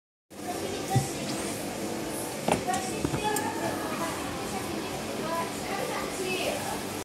How I Printed My Shard and Borough Market Screen Print
Watch the screen printing process - here's the first layer, the yellow colour going down.